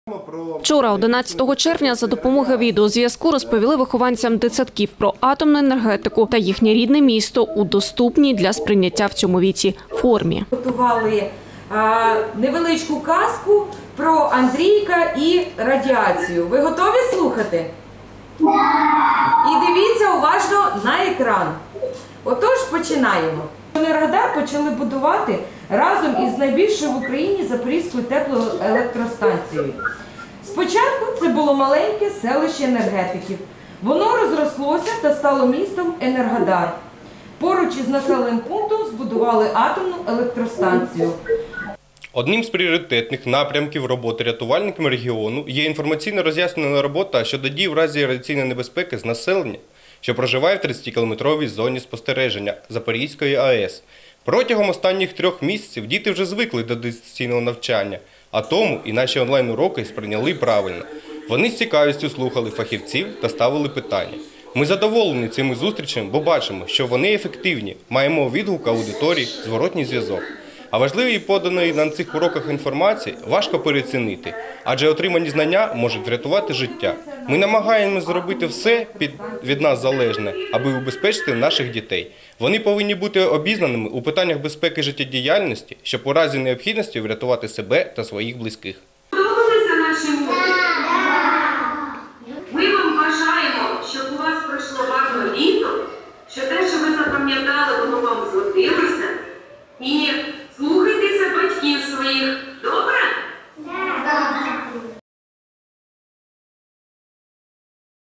Новини - Урок ДСНС для дітей - 12.06.2020
Вчора, 11 червня, за допомоги відеозв*язку, розповіли вихованцям дитсадків про атомну енергетику та їхнє рідне місто у доступній для сприйняття в цьому віці формі.